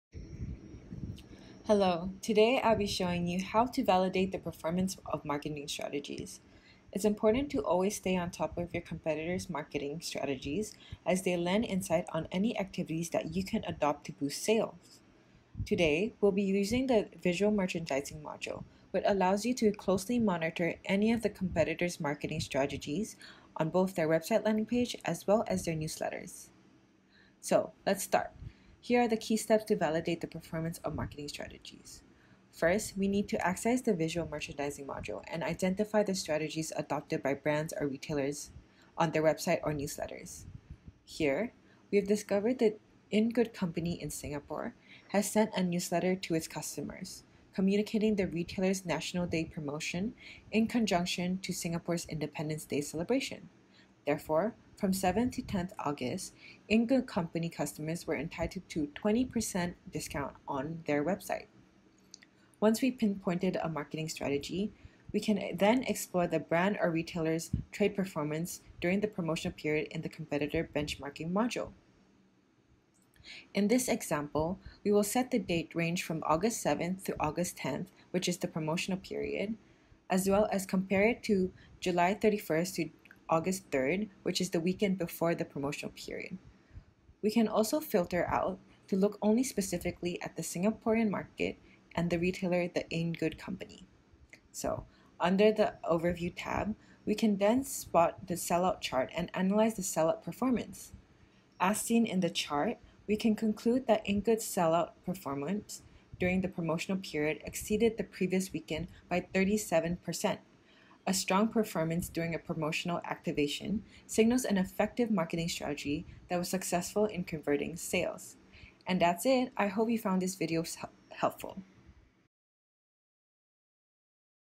How to Benchmark Competitors and Validate the Performance of Marketing Strategies | Omnilytics
"The Omnilytics Visual Merchandising module allows for close monitoring of your competitors’ marketing strategies on their website landing pages and newsletters. Information can be filtered by market, retailer, category and keyword to quickly narrow down your search."

In this video, we teach you the key steps in validating the performance of marketing strategies using our retail intelligence software.

In an increasingly competitive landscape, fashion brands and retailers must always stay on top of their competitors' marketing activities to stay ahead of the game. Benchmarking for competitive advantage will help build impactful strategies that accelerate sales.

Step 1: Access the Visual Merchandising module and identify the strategies adopted by brands or retailers on their website or newsletter
Step 2: Compare trade performance during promotional period against a previous period in the Competitor Benchmarking module

The stakes are now higher as more fashion brands and retailers scramble to grab market share. By learning how to benchmark competitors using Omnilytics' tools, you will be able to gain relevant ideas on the right tactics to deploy for your own business and accelerate sales.